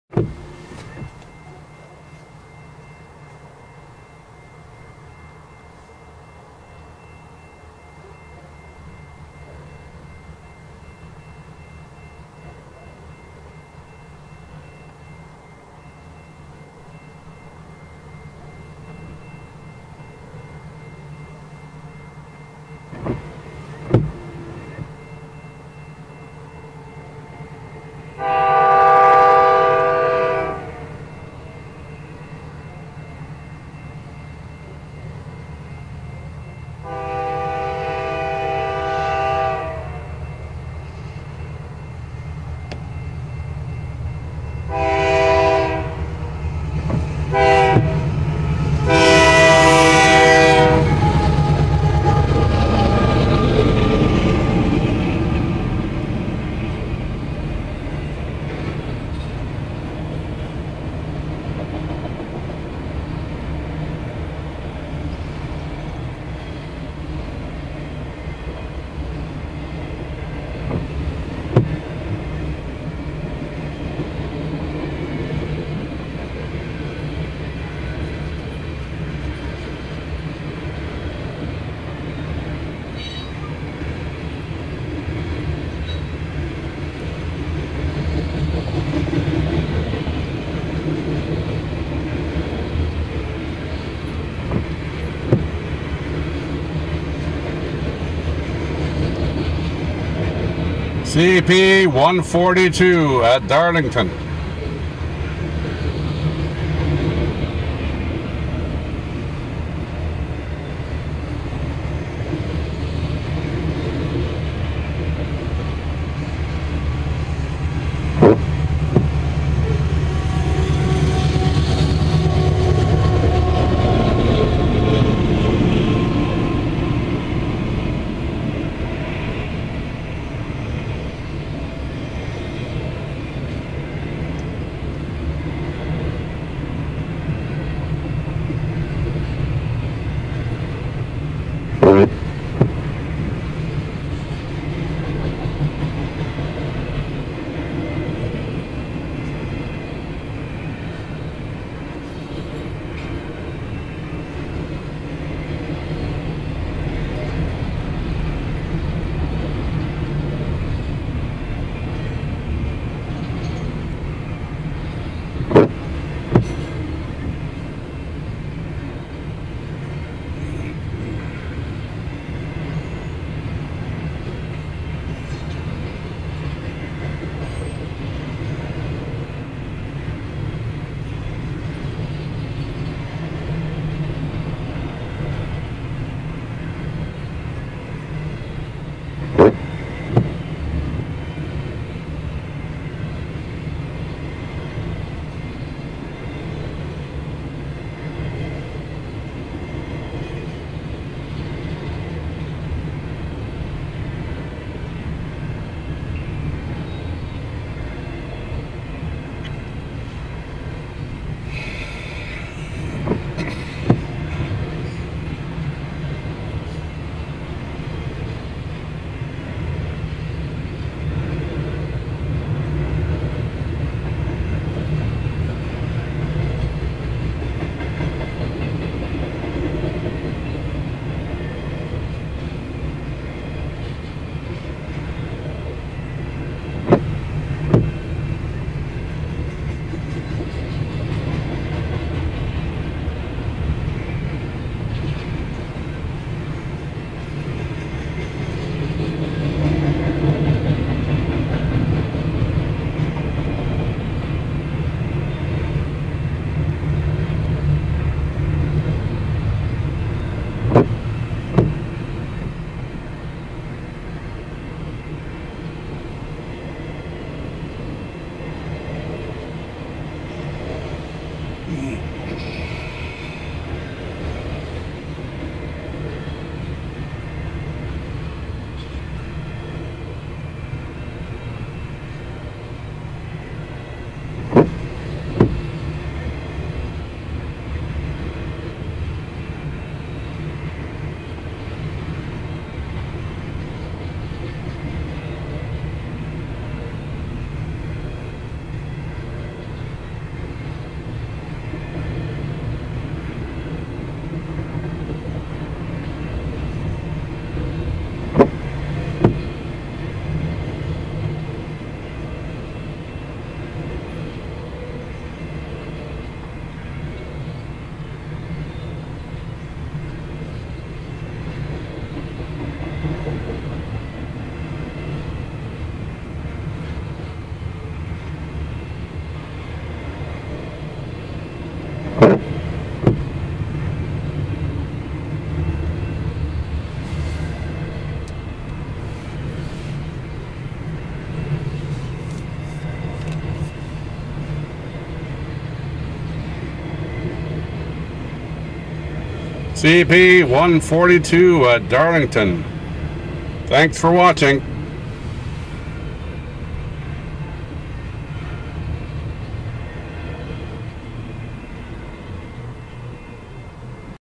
CP #142/ CP #8563, DPU CEFX#1055/ DARLINGTON
CP#142/ CP AC4400CW#8563, DPU CEFX AC4400CW#1055 / MIXED / INTERMODAL /AT DARLINGTON , MILE 169.31 BELLEVILLE SUB:(02/25/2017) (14:24hr.)